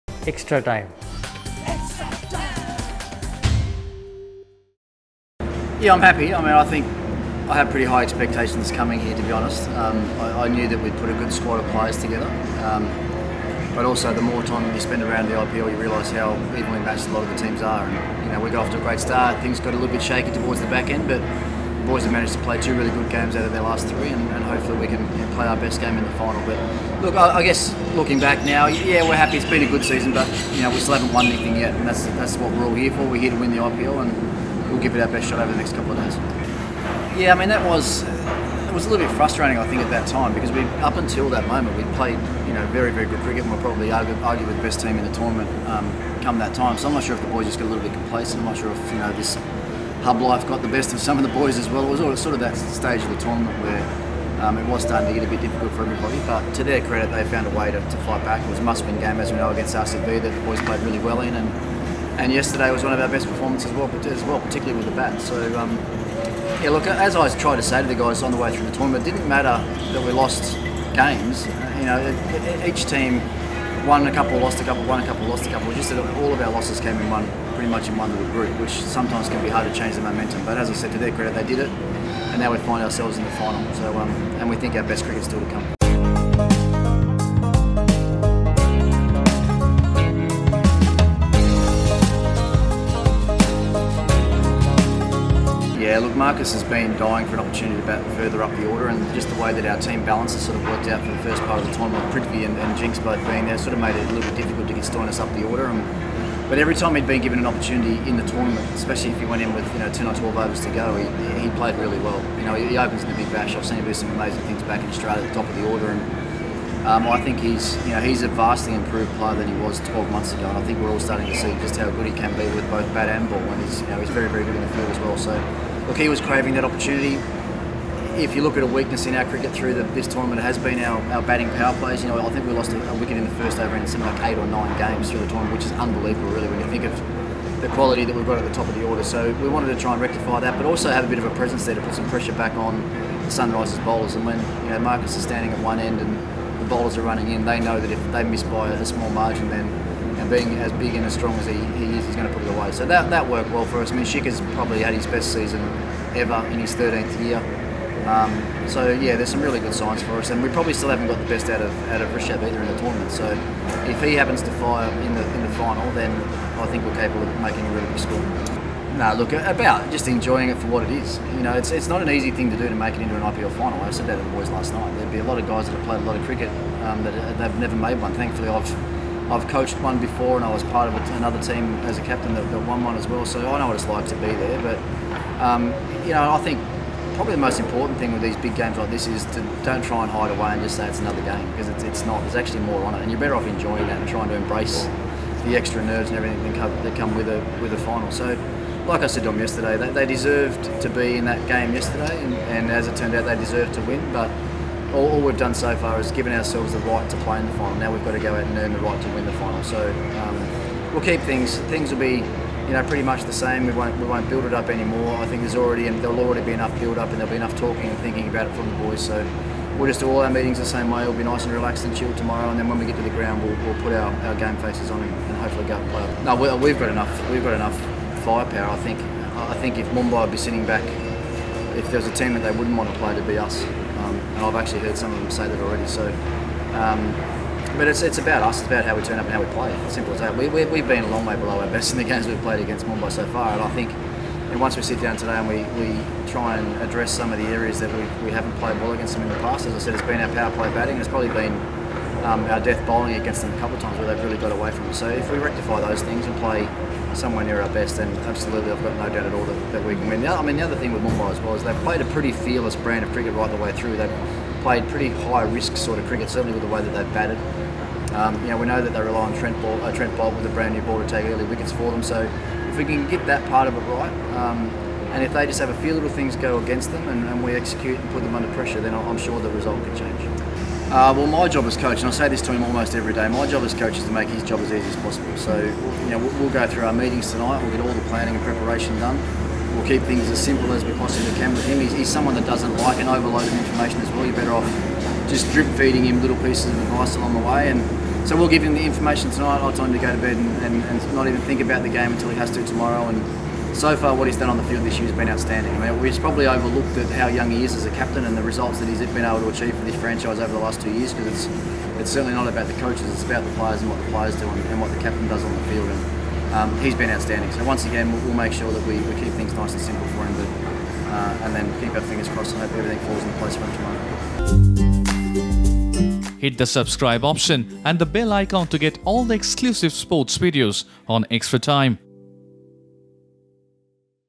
IPL 2020: We are here to win the final, says Delhi Capitals Coach Ricky Ponting ahead of final vs MI
Story Editor: Manoj Jana

Delhi Capitals Head Coach Ricky Ponting is confident ahead of the IPL 2020 final against Mumbai Indians in Dubai